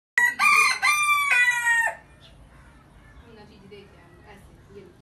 animal cock
This is a cock that is calling out loud, its voice is very melodious, this is a breathy cock, this is a very good cock.This video is made for entertainment purposes, please comment on how you liked this video.